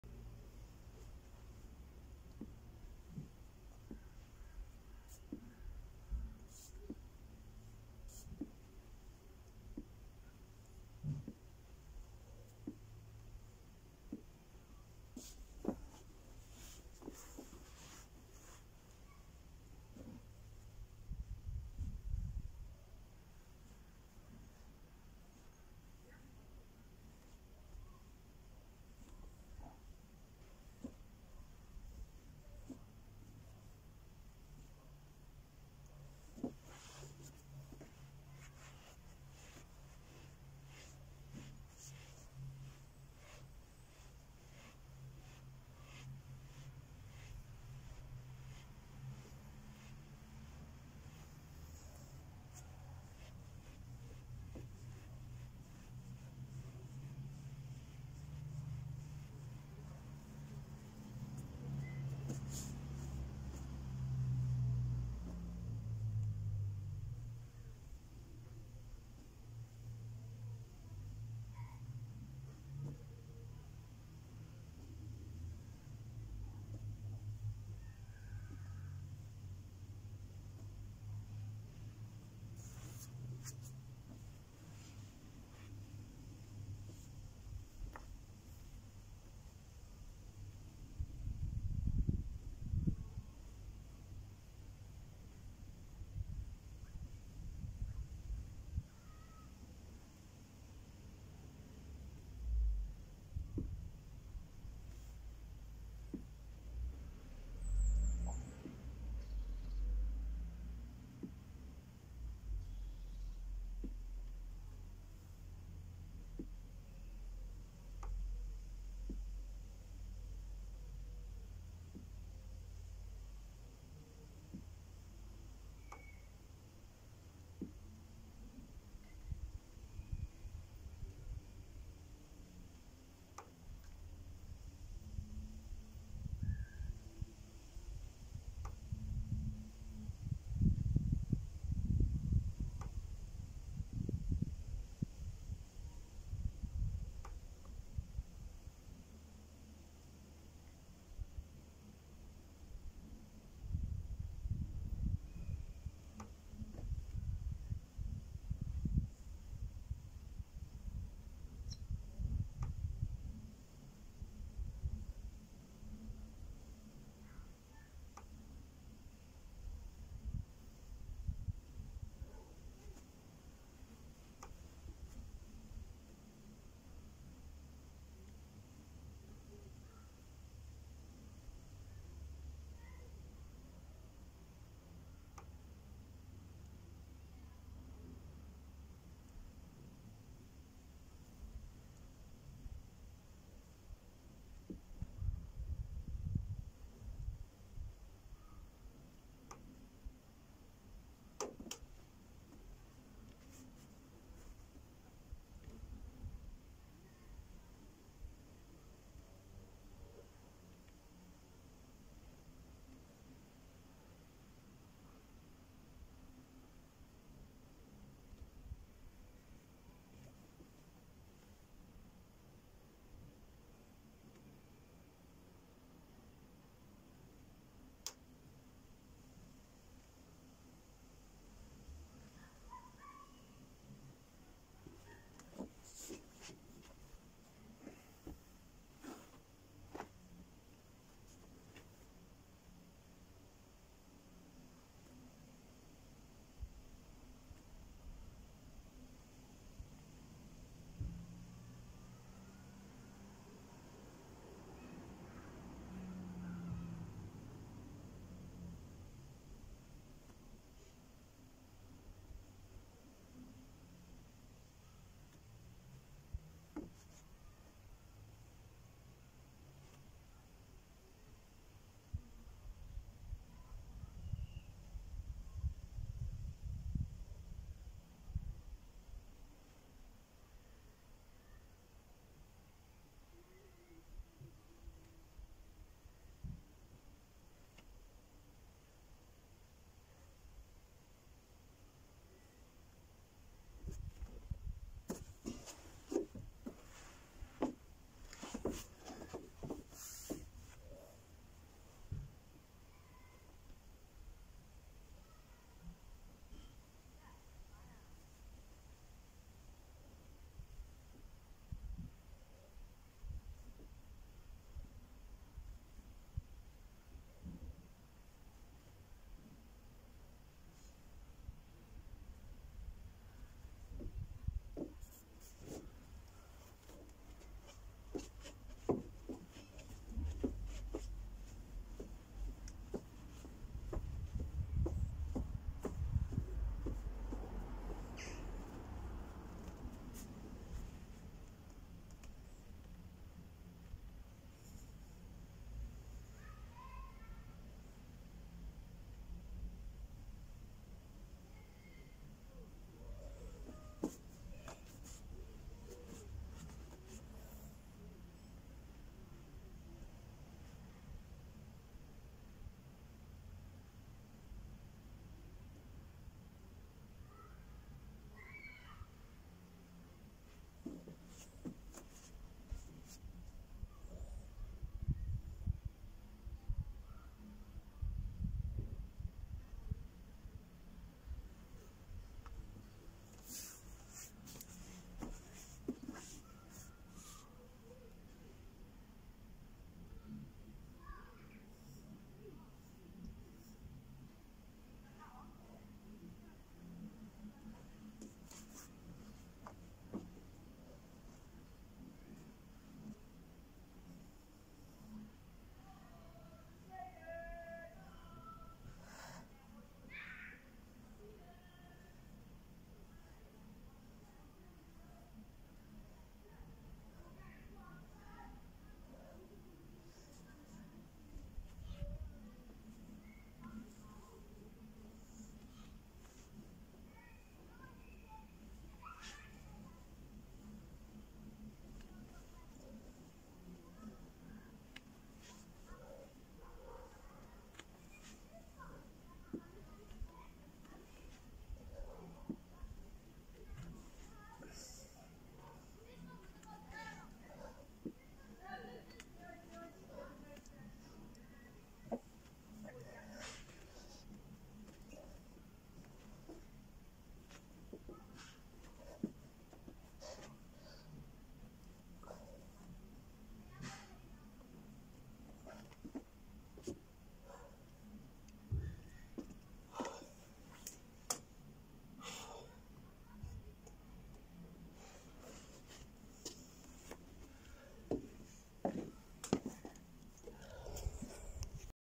Core Workout
My workout which will get you fired up before breakfast! Takes less than 8 minutes unless you want to increase your intensity by adding more repetitions or even slowing them down creating more time under tension. I'll be uploading more with better camera angles.